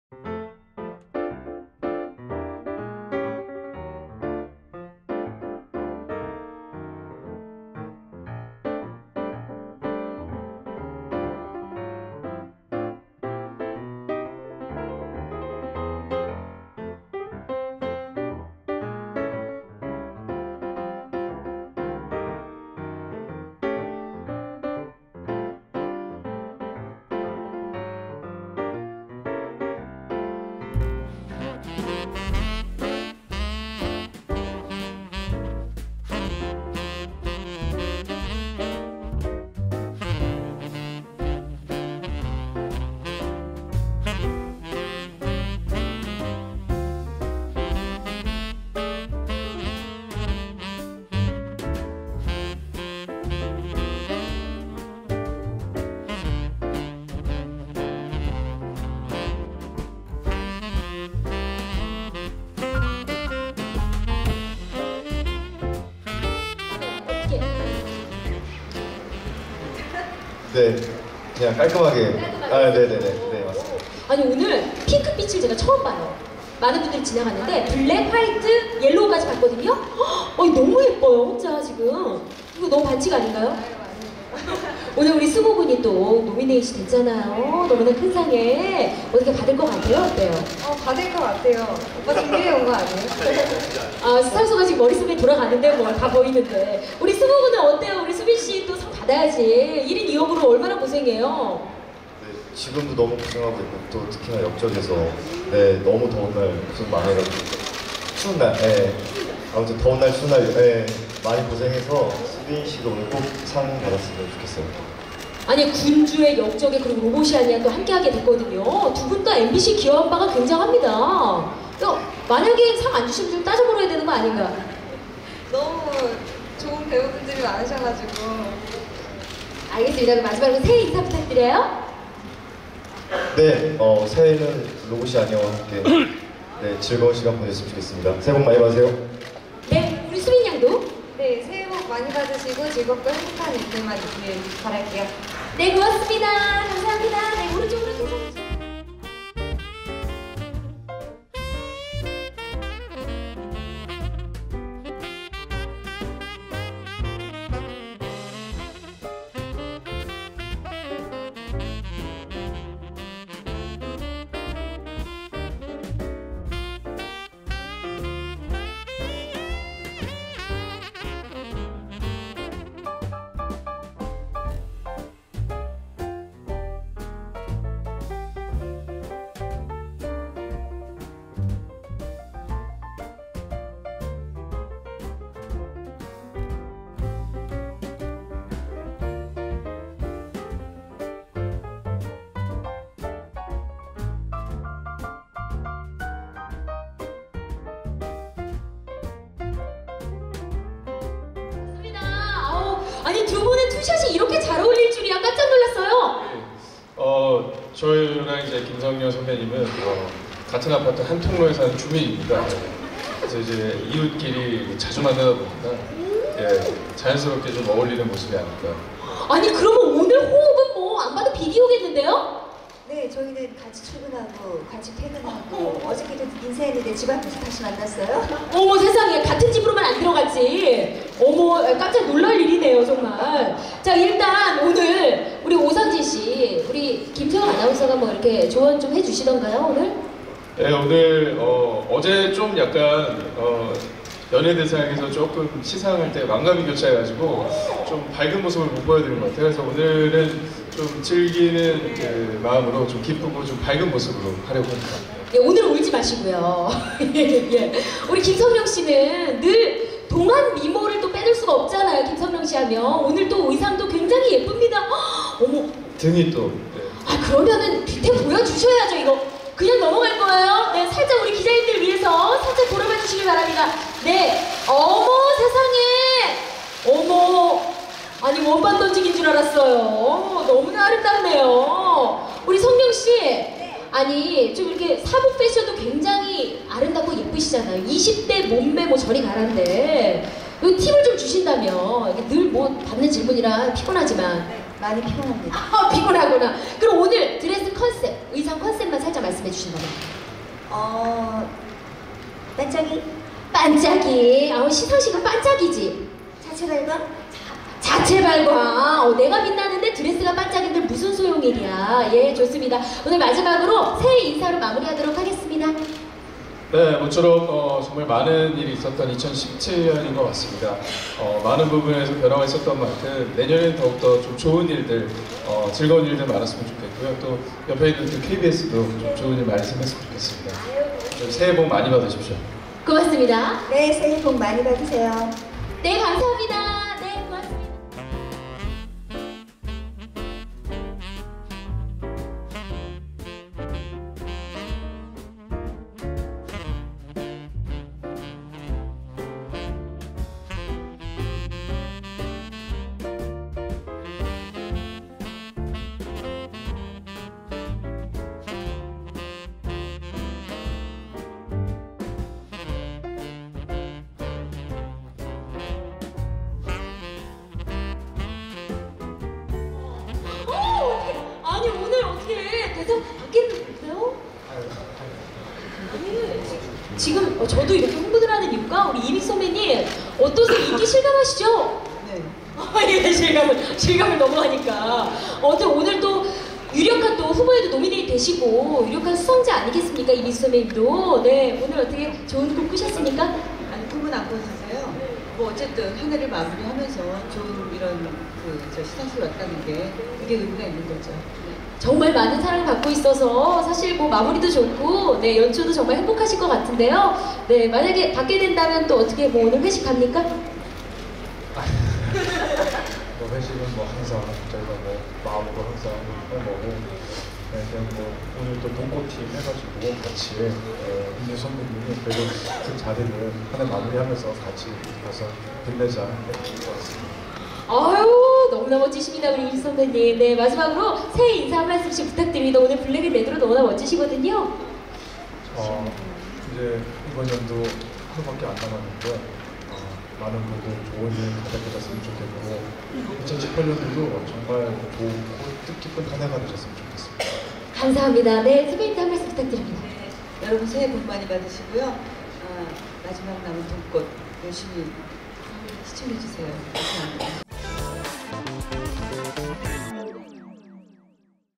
레드카펫을 비추는 선남선녀 커플들 장혁&이미숙, 김성령&오상진, 유승호&채수빈, 이선빈&오정세
매주 수요일 오후 6시 30분   tbs TV 본방송